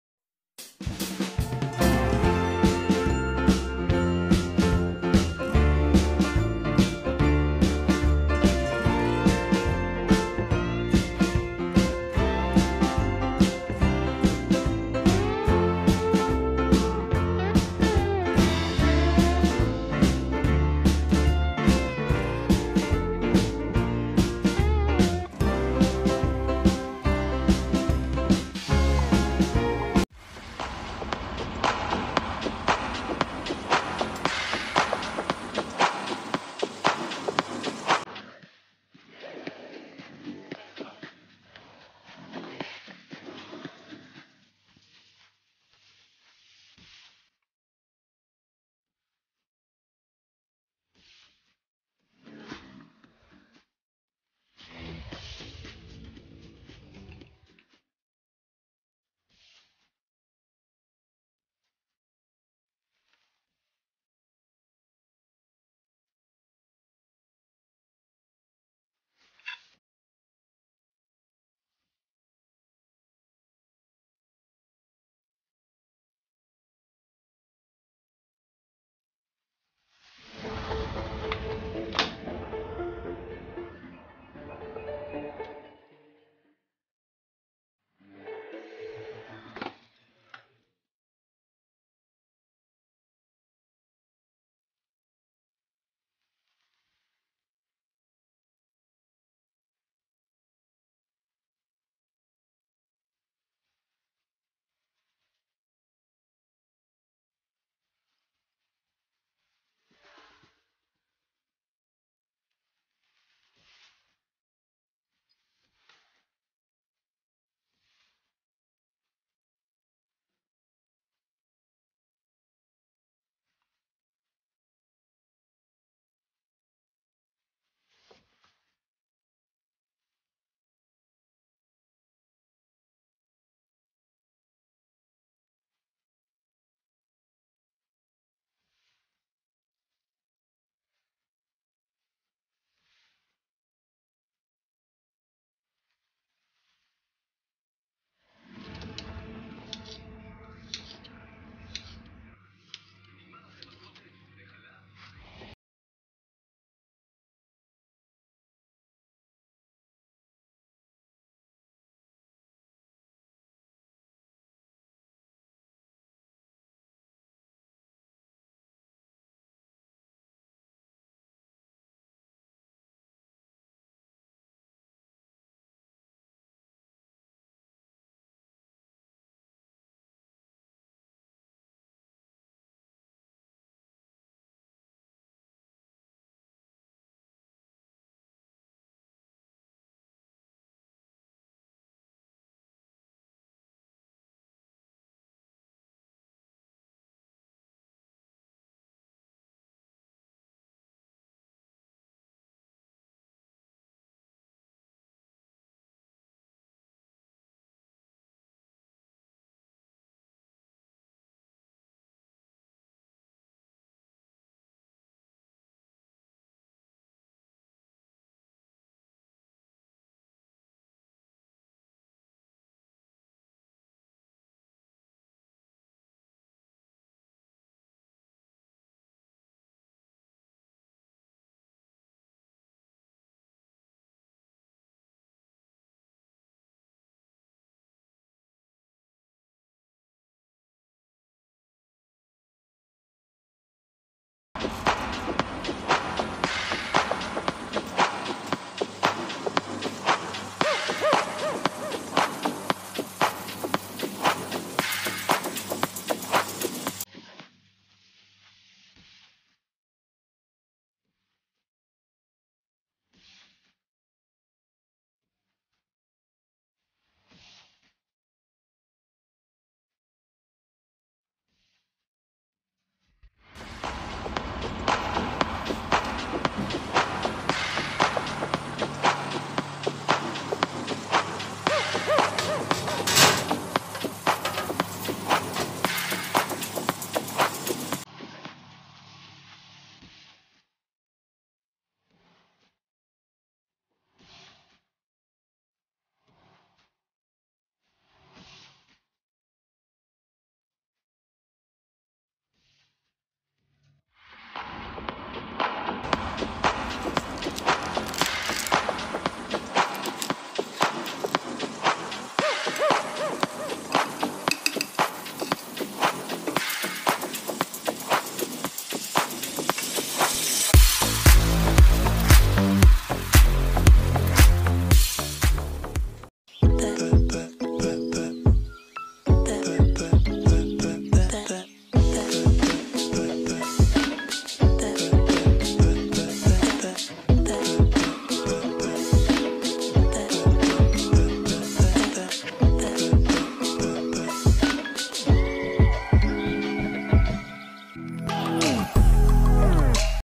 FISFFILLET WITH GARLIC AND PARSLEY
Fishfillet with parsley and garlic
Simple gourmet fish roasted in oven
Yummy dish.